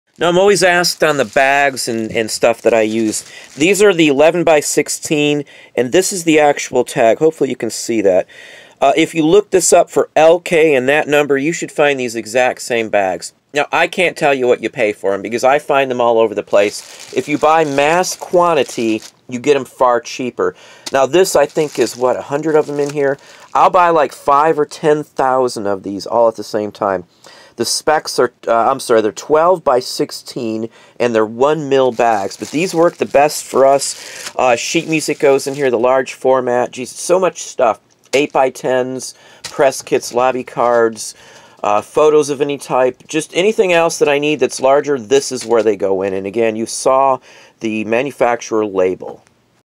Cheap & Effective Shipping Supplies
Some of the Mailing Supplies We Use:
2" Table Top Tape Dispenser
2” Tape For Dispenser Above
6" X 9" poly mailing bags
10” X 14” & Larger Poly Mailing Bags